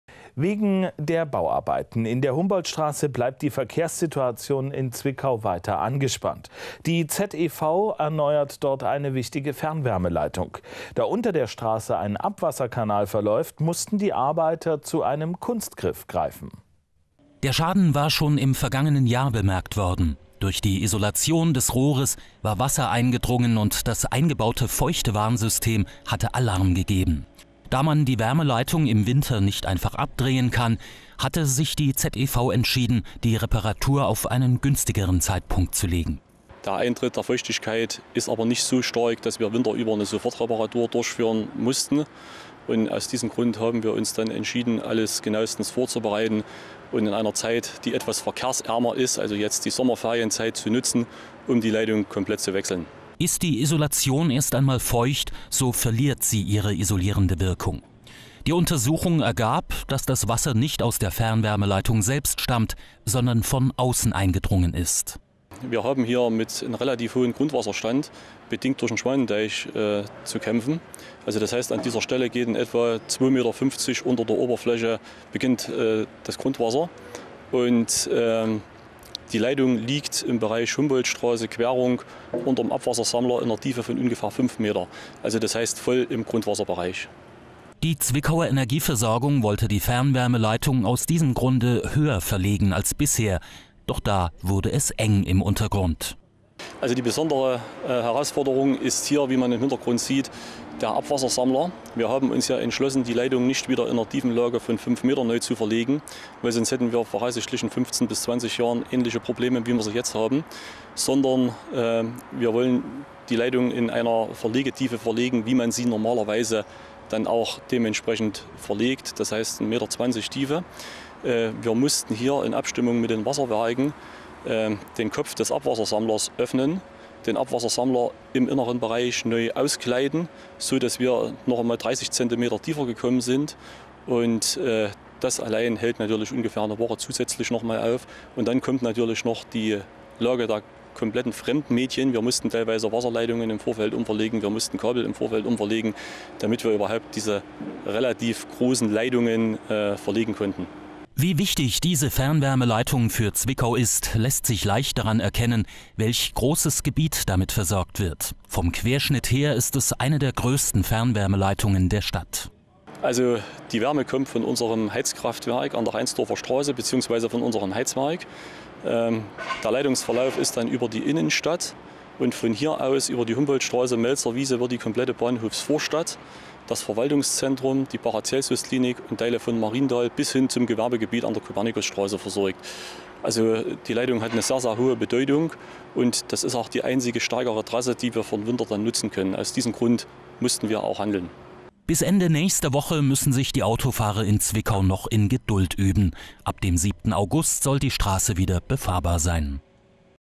Humboldtstraße noch knapp zwei Wochen dicht
Wegen der Bauarbeiten in der Humboldtstraße bleibt die Verkehrssituation in Zwickau angespannt. Die ZEV erneuert dort eine wichtige Fernwärmeleitung. Da unter der Straße ein Abwasserkanal verläuft, mussten die Arbeiter einen Kunstgriff anwenden.